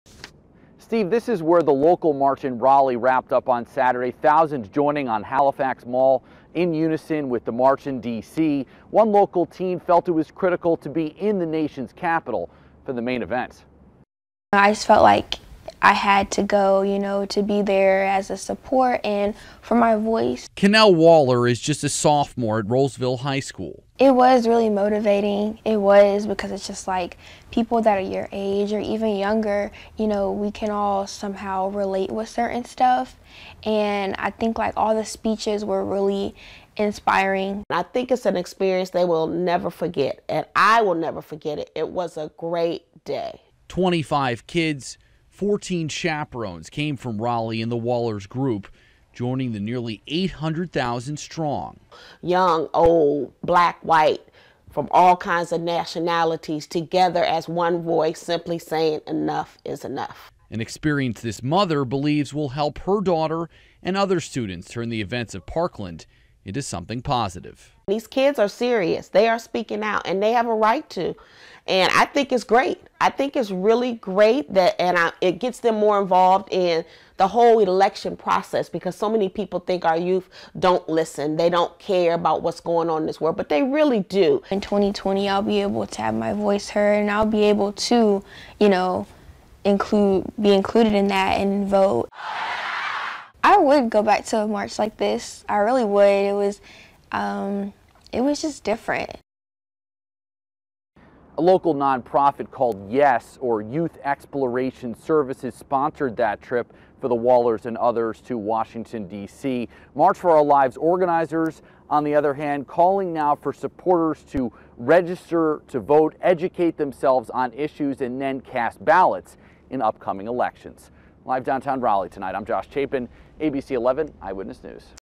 Wake teen, mother reflect on March for our Lives rally
A teenager reflects on the March for Our Lives Rally